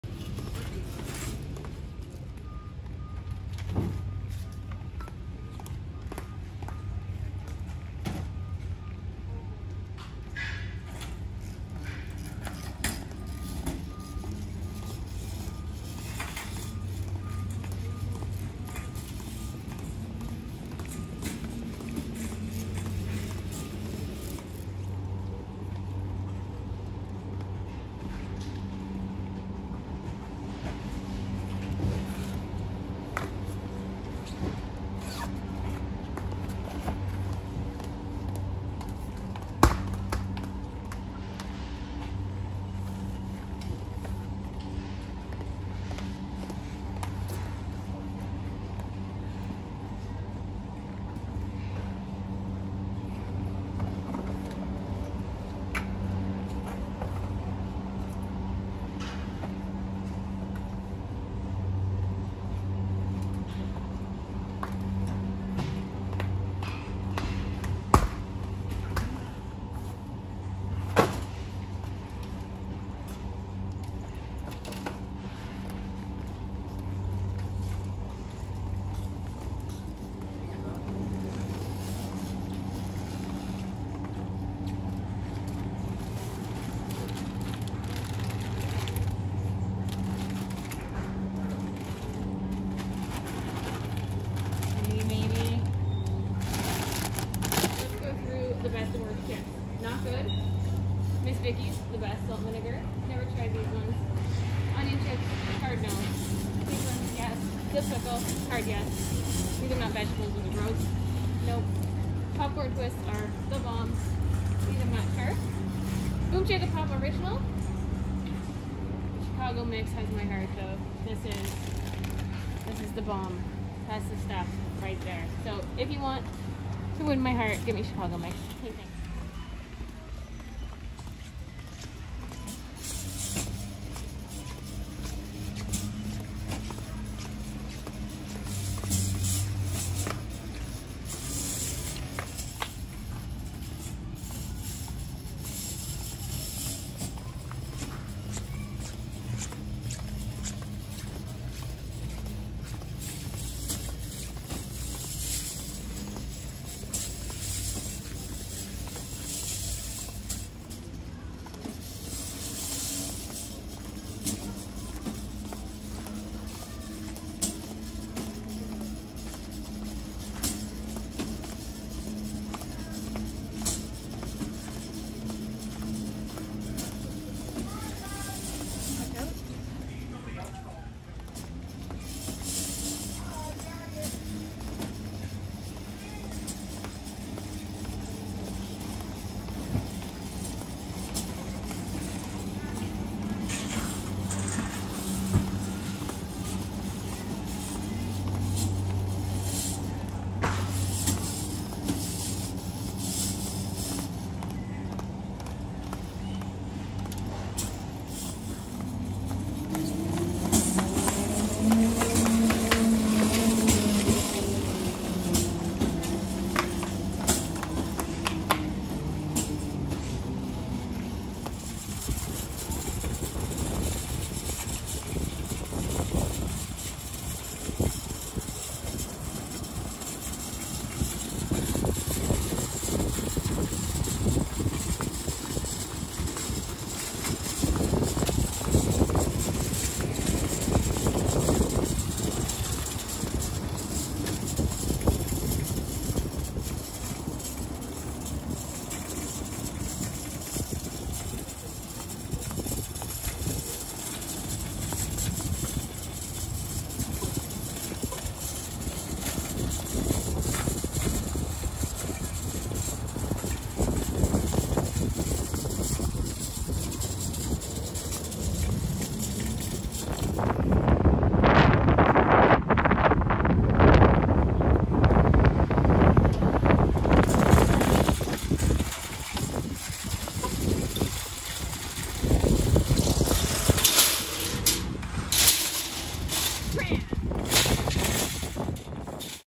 Let’s go shopping - Costco! Outfit - Jean shorts and crop top and heels
I’m back home for a minute so of course I go to Costco to load up on my favourites…
Starbucks Iced coffee 💁🏼‍♀️ What else can you spot? Besides the cart getting TOO heavy!
*Shot before mask restrictions were lifted in my region. Don’t be a baby

for my VIP page, Socials, and About Me!